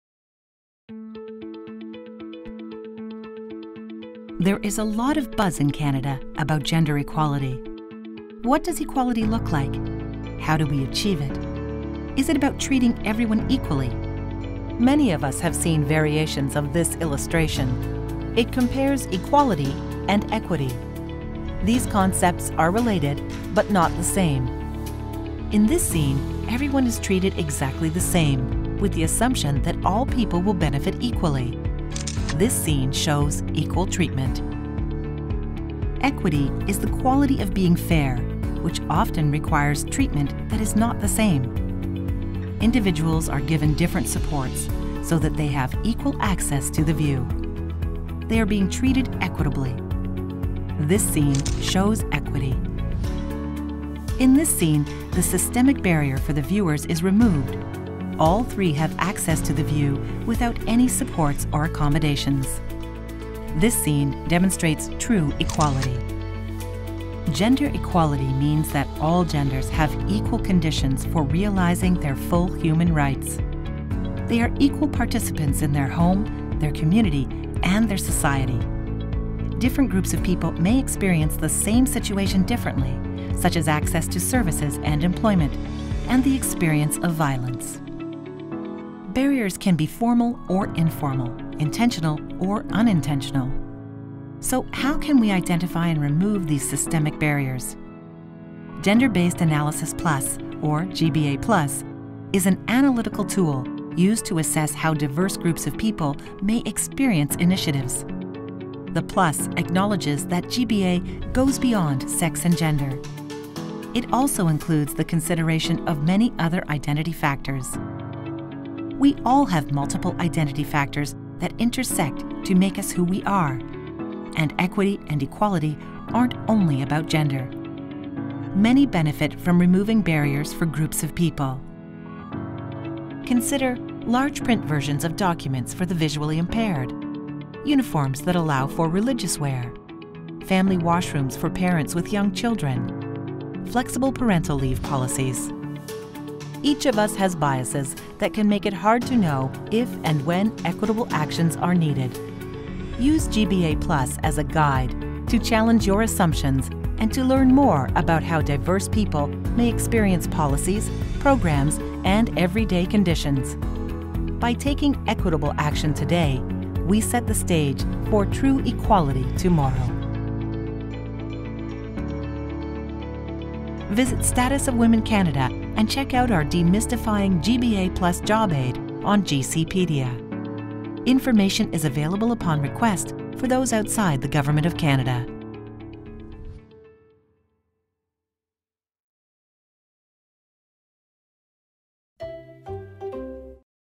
GBA+: Equality or Equity?
There is a lot of buzz in Canada about gender equality.  What does equality look like? How do we achieve it?  Is it about treating everyone equally?

Many of us have seen variations of this illustration.  It compares equality and equity.

These concepts are related, but not the same.

In this scene, everyone is treated exactly the same, with the assumption that all people will benefit equally.  This scene shows equal treatment.

Equity is the quality of being fair, which often requires treatment that is not the same.   Individuals are given different supports so that they have equal access to the view.  They are being treated equitably.  This scene shows equity.

In this scene, the systemic barrier for the viewers is removed.  All three have access to the view without any supports or accommodations.  This scene demonstrates true equality.

Gender equality means that all genders have equal conditions for realizing their full human rights.

They are equal participants in their home, their community and their society.

Different groups of people may experience the same situation differently, such as access to services and employment, and the experience of violence.

Barriers can be formal or informal, intentional or unintentional.

So, how can we identify and remove these systemic barriers?

Gender-based Analysis Plus (or GBA+) is an analytical tool used to assess how diverse groups of people may experience initiatives.

The “plus” acknowledges that GBA goes beyond sex and gender. It also includes the consideration of many other identity factors.

We all have multiple identity factors that intersect to make us who we are.  And, equity and equality aren’t only about gender.

Many benefit from removing barriers for groups of people.  Consider …

• Large print versions of documents for the visually impaired
• Uniforms that allow for religious wear
• Family washrooms for parents with young children
• Flexible parental leave policies

Each of us has biases that can make it hard to know if and when equitable actions are needed.   Use GBA+ as a guide, to challenge your assumptions and to learn more about how diverse people may experience policies, programs and everyday conditions. By taking equitable action today, we set the stage for true equality tomorrow.

Visit Status of Women Canada and check out our Demystifying GBA+ job aid on GCpedia.

Information is available upon request for those outside the Government of Canada.